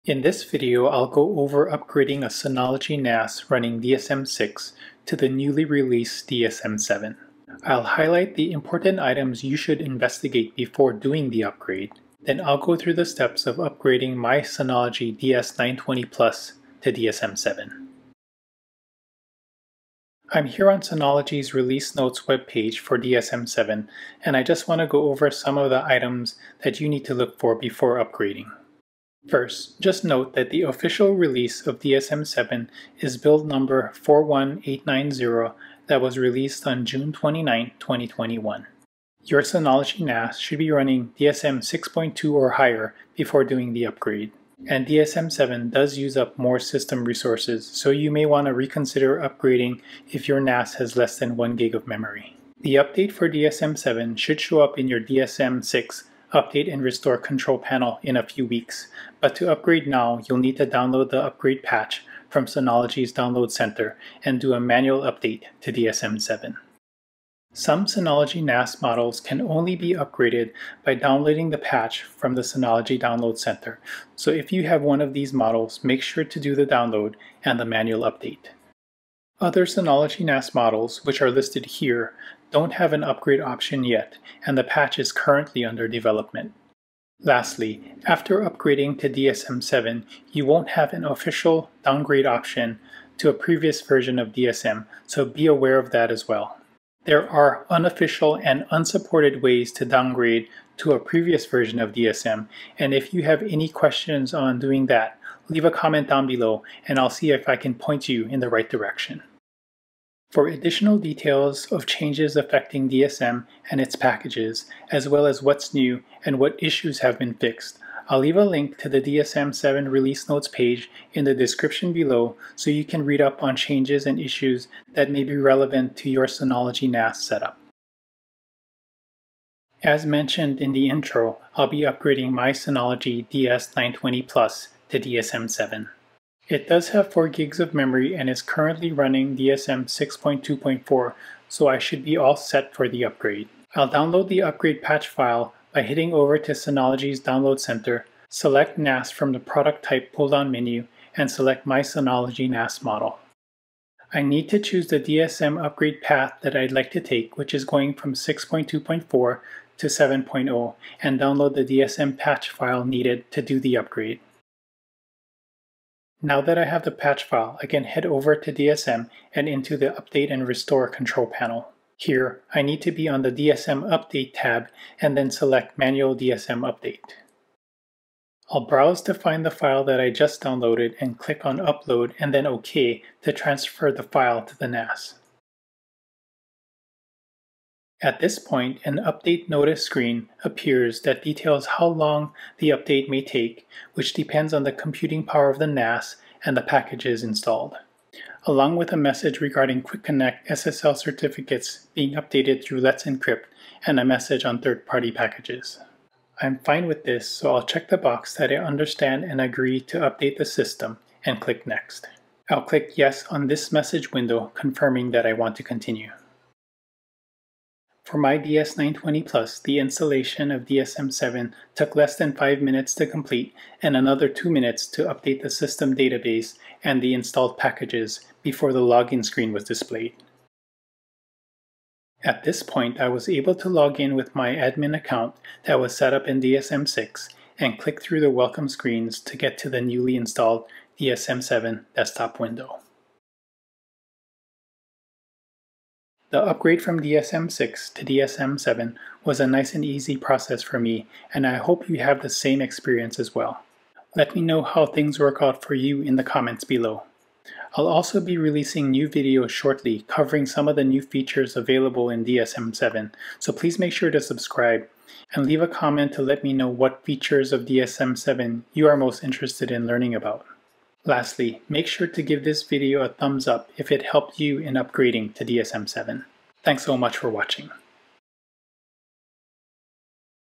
Upgrade A Synology NAS Running DSM 6 To DSM 7
This video covers upgrading a Synology NAS running DSM 6 to the newly released DSM 7.

The video topics include:
• Highlighting important items to investigate before upgrading a Synology NAS from DSM 6 to DSM 7.
• Step by step walk through of upgrading a Synology DS920+ running DSM 6 to DSM 7.

• Synology 2 Bay NAS DS220+
• Synology 2 Bay NAS DS720+
• Synology 4 Bay NAS DS920+

Timecodes
0:00 | Introduction
0:19 | Before Upgrading To DSM 7 Notes
2:10 | Synology DS920+ Specs And DSM 7 Patch Download
2:45 | Update Synology DS920+ To DSM 7
3:52 | Login To Upgraded Synology DS920+ Running DSM 7
4:05 | Closing